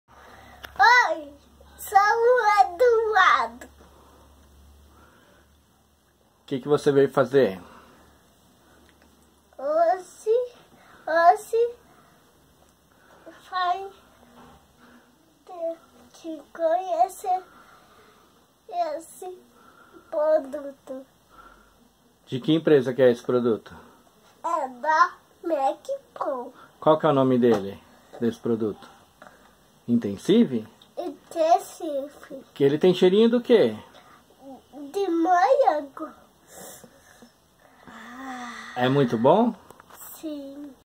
Kit Intensive Macpaul, hidratação profunda.
O kit Intensive é ideal para o uso freqüente. Sua fórmula equilibrada, rica em agentes hidratantes, trata os fios e proporciona uma limpeza suave. Possui uma espuma rica e cremosa que hidrata e condiciona os cabelos, diminuindo o atrito entre os fios. Devolve a maleabilidade, o brilho e a vitalidade natural aos cabelos. Seus agentes hidratantes formam um filme,que protege os fios e ajuda a reter a hidratação.